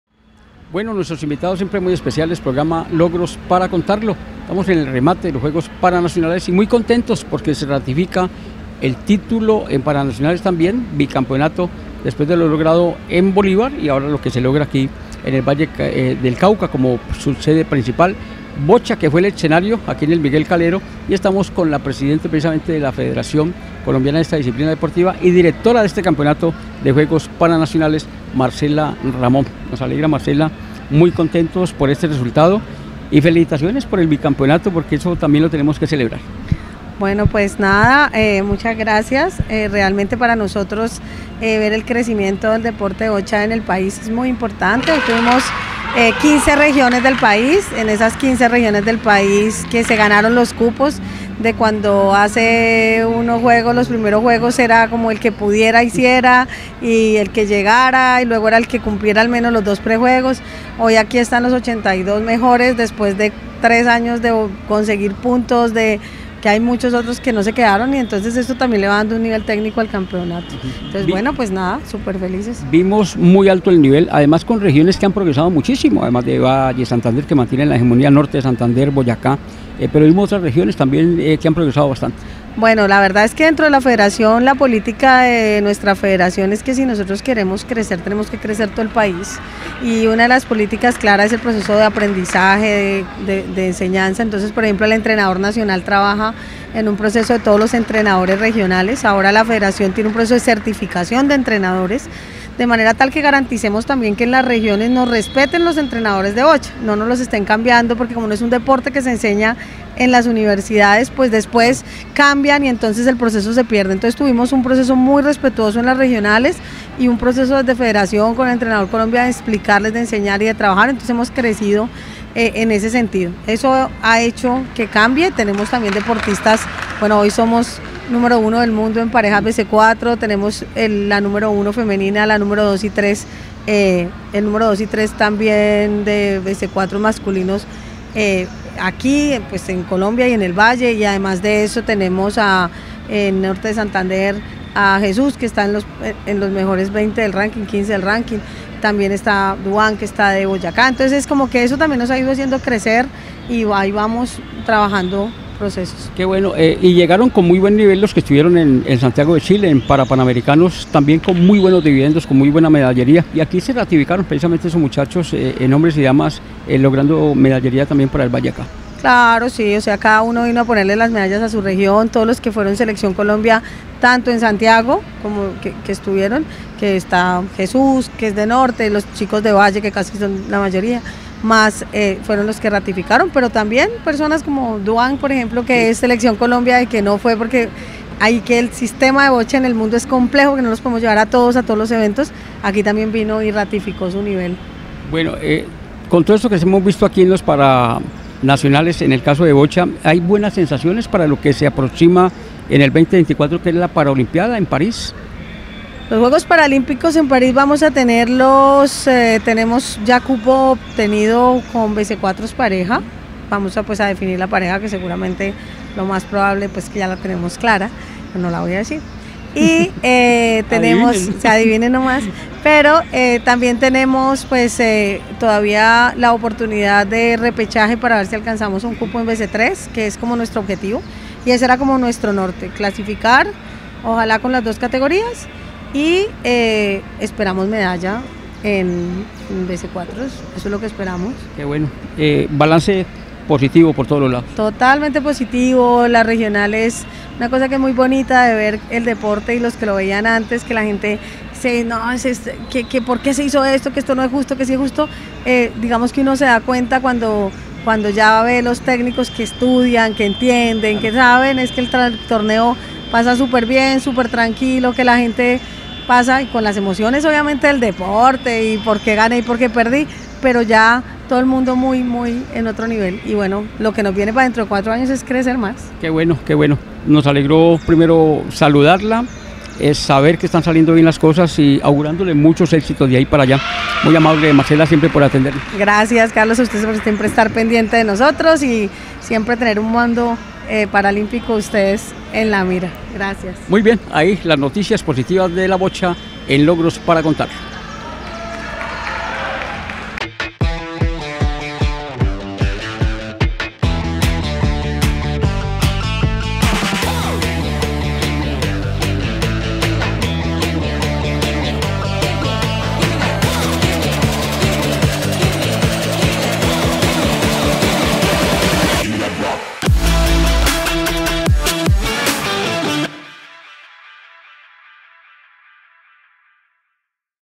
Marcela Ramón Cuellar, Presidente Federación Colombiana de Boccia Juegos Paranacionales 2023
XXII Juegos Deportivos Nacionales y V Paranacionales Eje Cafetero - La Boccia en los Juegos Paranacionales de Colombia es una emocionante disciplina deportiva que brinda la oportunidad a atletas con discapacidades físicas o motoras de participar y competir a nivel nacional. La Boccia es un juego de precisión y estrategia, similar a la petanca, en el que los deportistas lanzan bolas blandas con el objetivo de acercarlas lo más posible a una bola objetivo, conocida como "jack". La competición se lleva a cabo en una cancha especial, y los jugadores deben demostrar habilidades tanto físicas como mentales para calcular lanzamientos precisos y tácticas inteligentes. - Los Juegos Paranacionales son una plataforma inclusiva que fomenta la participación y el desarrollo de talentos en la Boccia, promoviendo la igualdad de oportunidades para personas con diversidad funcional. Los atletas representan a sus regiones y compiten en diferentes categorías, según el grado de discapacidad, lo que permite una competición justa y equitativa. Estos juegos no solo destacan el talento deportivo de los participantes, sino que también impulsan la conciencia y el respeto hacia las personas con discapacidad, creando un ambiente de integración y admiración por la capacidad humana de superar desafíos a través del deporte.

Transcripción